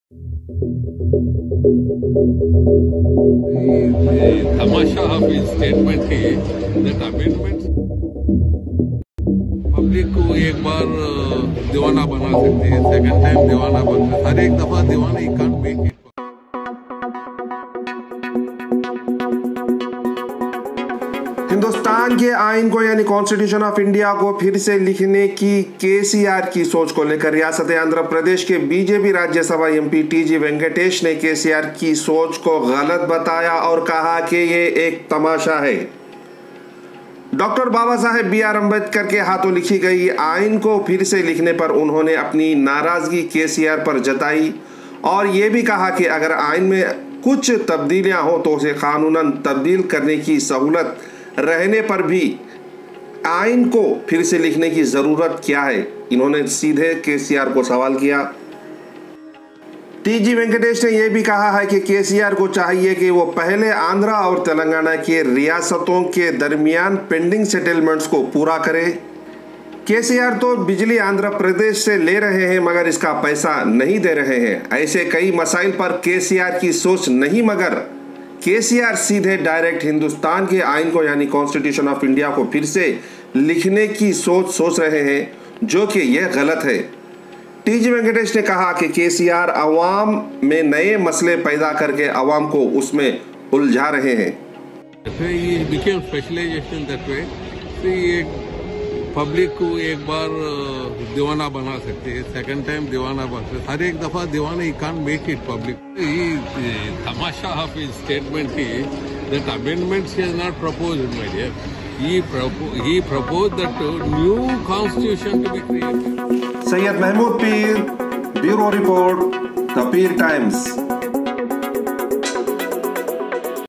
T.G.V.Says "TAMASHA" K.C.R. Ka - The Peer Times YouTube Channel
T.G. Venkatesh Member of Parliament (Rajya Sabha) says KCR doing Tamasha and he says first he has to settle the settlements which were due between Andhra and Telangana and